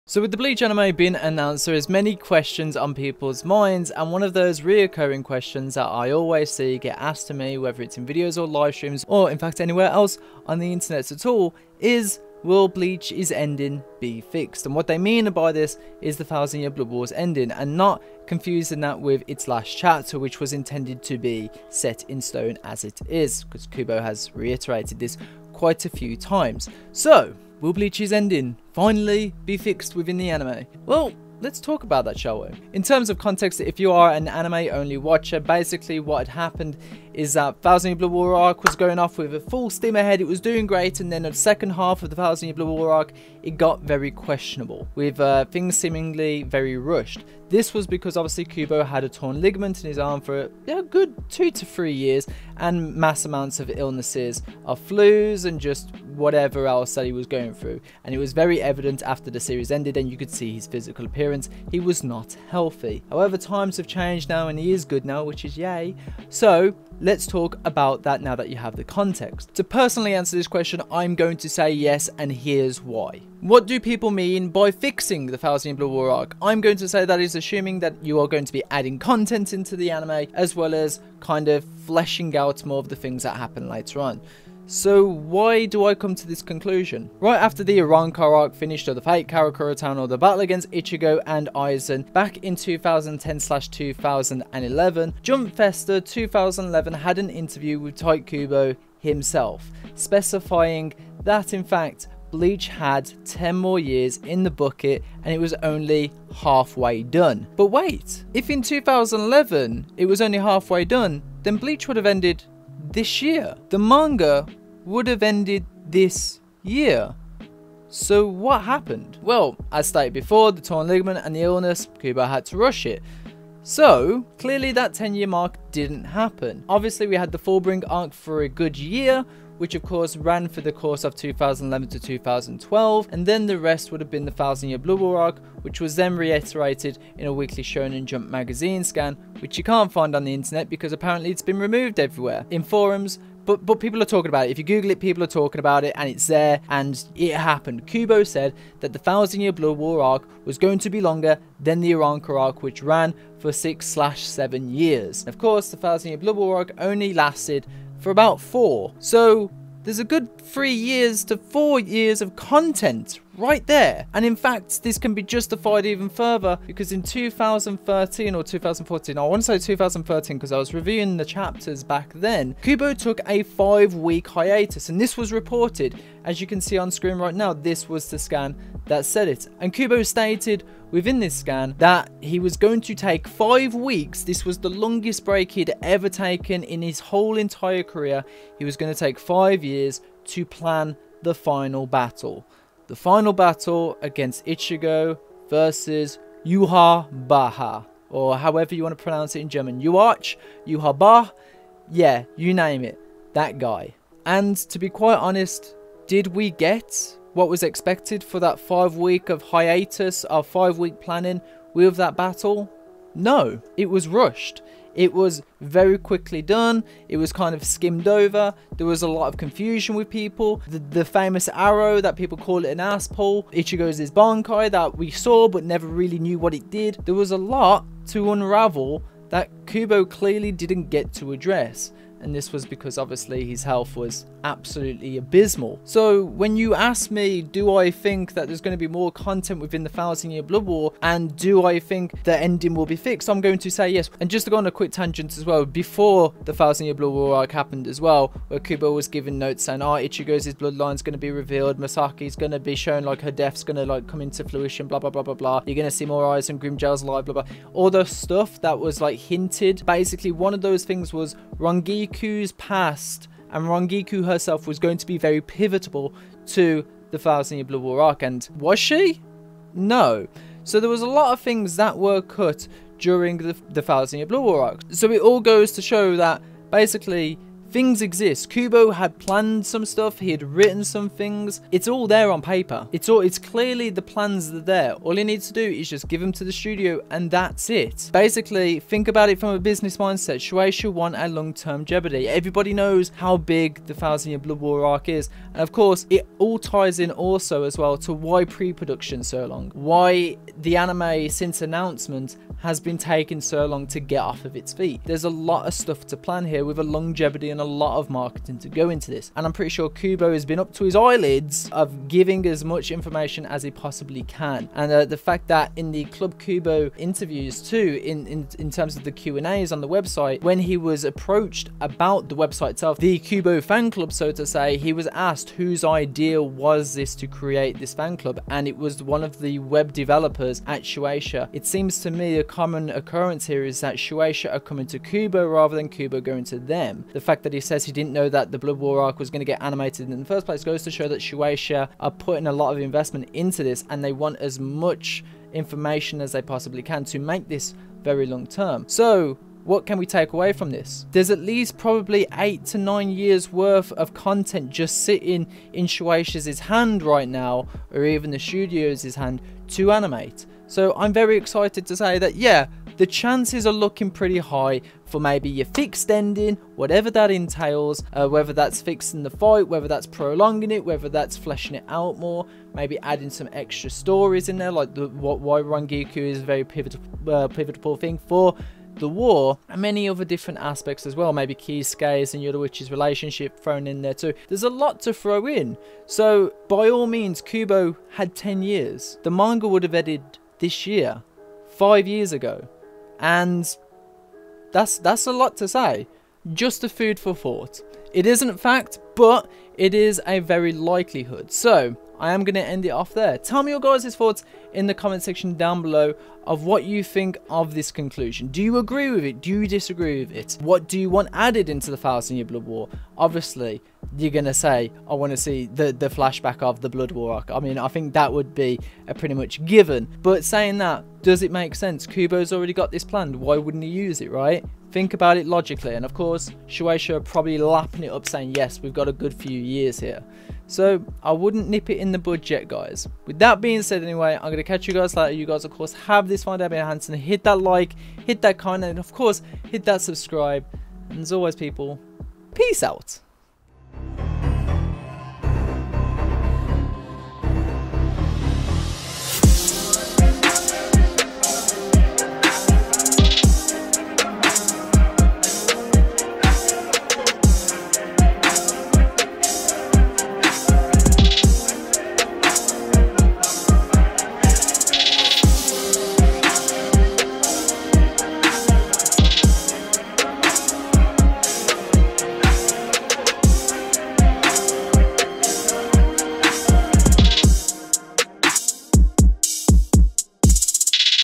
Will Bleach’s Anime Ending Change?
The long-awaited question... Will the Bleach Anime change its ending?... Well, let us discuss this once and for all!

- Bleach Analysis
- Bleach Discussions

SOCIALS